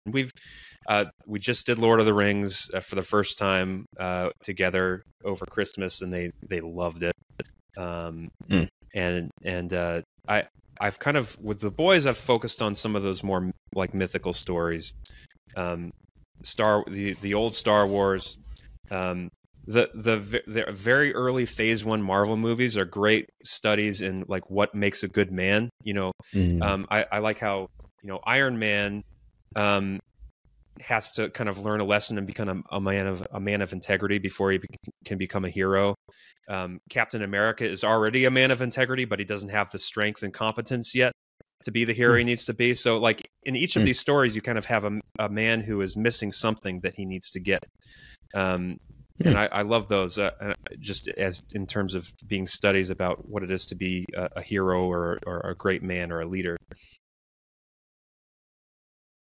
What Movies Can Teach Us
Dr. Owen Strachan and Joel Berry of the Babylon Bee come back together for another edition of the "Grace and Truth Movie Club!" They discuss various movies and their impact on Christians. They start by appreciating the storytelling and visuals of Dune Part II. Both Owen and Joel highlight the themes of leadership, courage, and the value of human life portrayed in these films. The conversation emphasizes the need for clear, decisive, and courageous men in today's society.